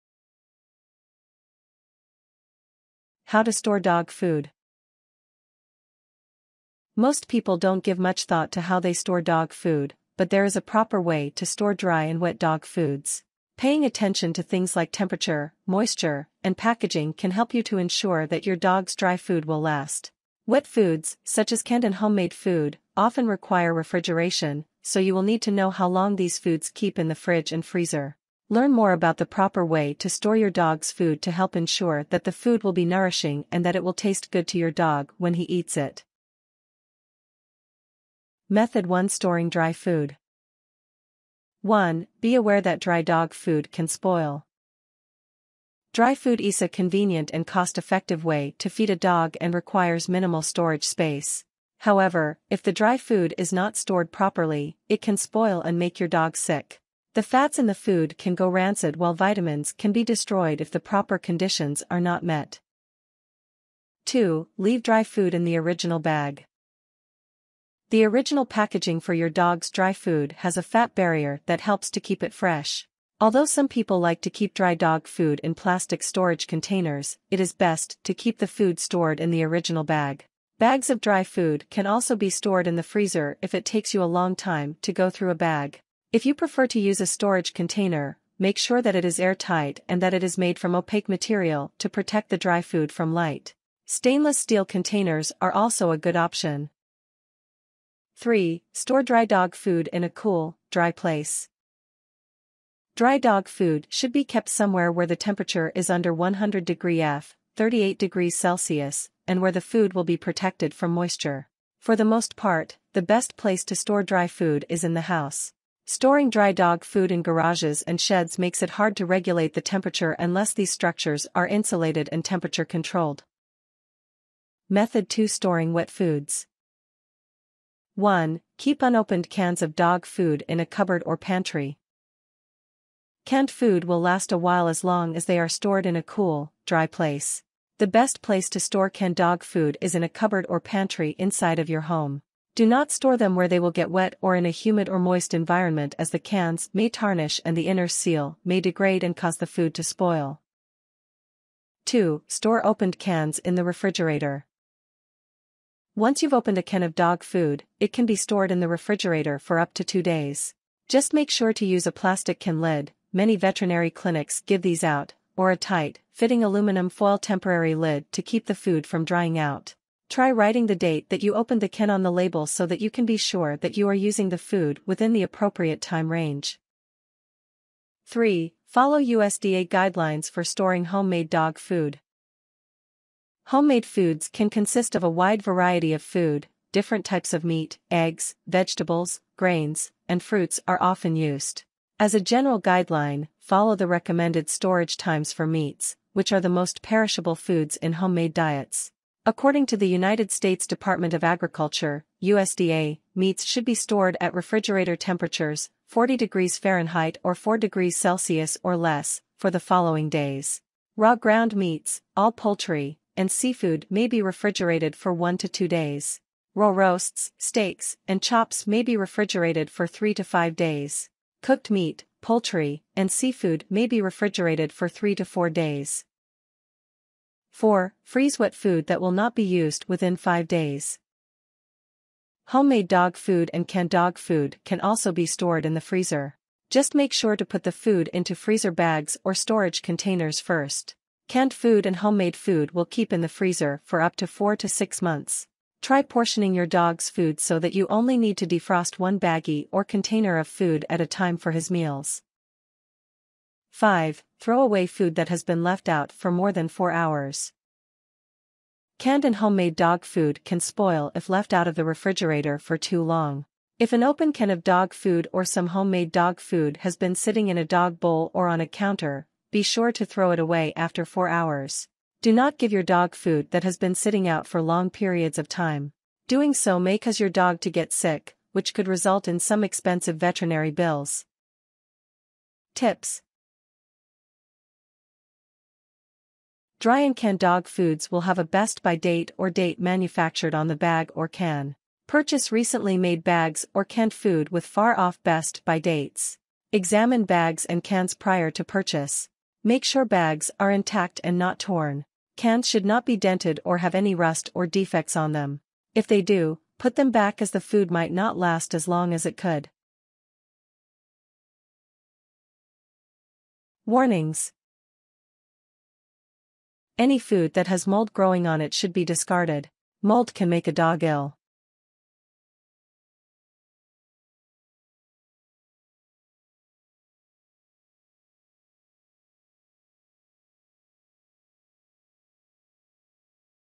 Proper Dog Food Storage Guide: Keep Your Furry Friend Safe & Healthy!
The video emphasizes the importance of proper pet food storage, particularly for dry and wet dog foods, to maintain freshness and ensure your dog's safety. For dry food, it's recommended to keep it in the original bag to preserve its fat barrier that prevents spoilage. However, if opting for storage containers, choose airtight, opaque ones, like stainless steel, to shield from light and air. It's crucial to store dry food in a cool, dry place, maintaining temperatures below 100°F (38°C), preferably indoors to regulate conditions effectively.    Regarding wet foods, unopened canned dog food should be stored in cool, dry cupboards or pantries, while opened cans can be refrigerated for up to two days, using proper lids to prevent drying. Homemade dog foods, encompassing various perishable ingredients, should adhere to USDA guidelines for storage times based on food types and refrigeration temperatures. Any unused homemade or canned food beyond five days should be frozen in suitable containers, allowing storage for up to four to six months.    An essential reminder is to discard any dog food left out for more than four hours, as it can spoil and pose health risks to your pet. Pay attention to "best by" dates when purchasing pet food, ensuring freshness, and examine bags and cans for any damage or defects that might compromise the food's quality. Finally, any dog food showing signs of mold should be discarded immediately, as mold can be harmful to your dog's health.